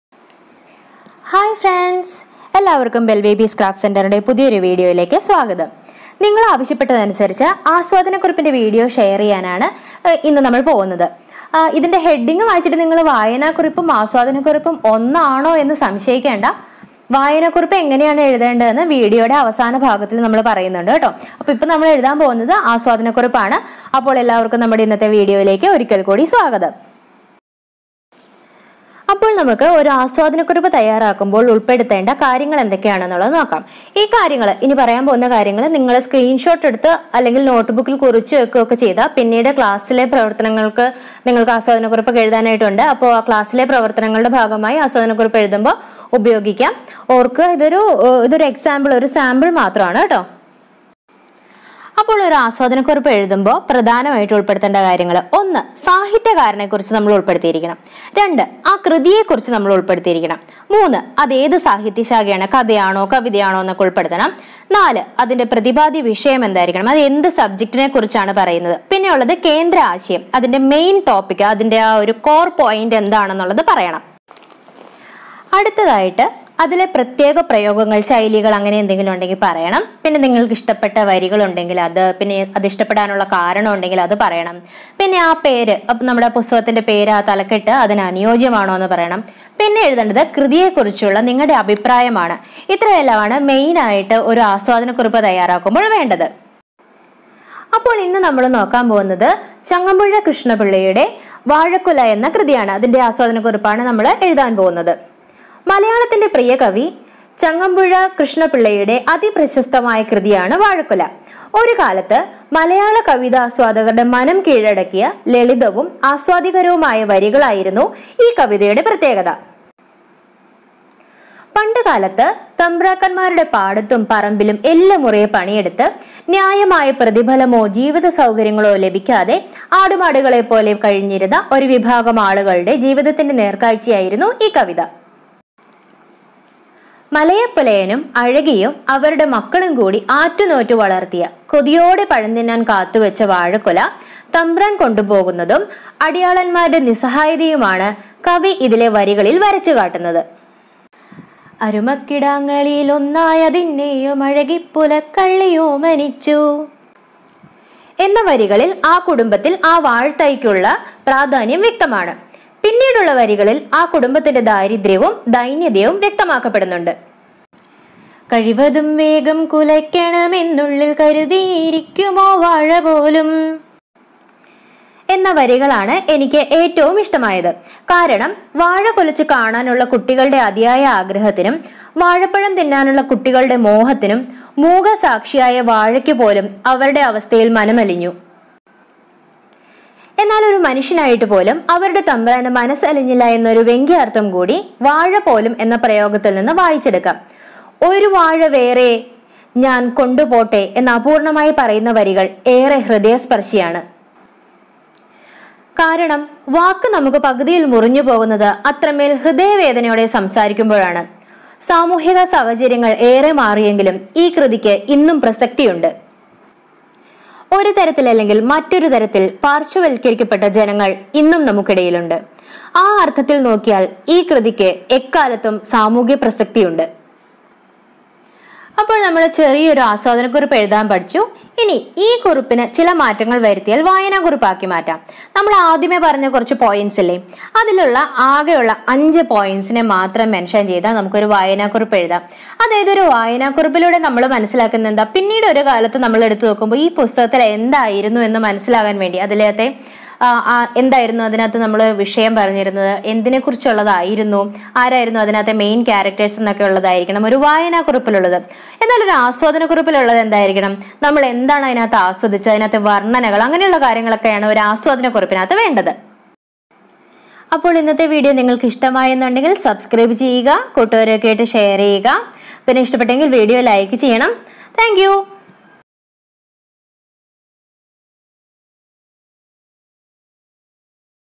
ആസ്വാദനക്കുറിപ്പ് തയ്യാറാക്കാം | UP and HS | വായനാദിന പ്രവർത്തനങ്ങൾ|June 19| Bellbaby's Craft Centre
In this video we have created a small note on a book that we have read. This is just a sample for your reference.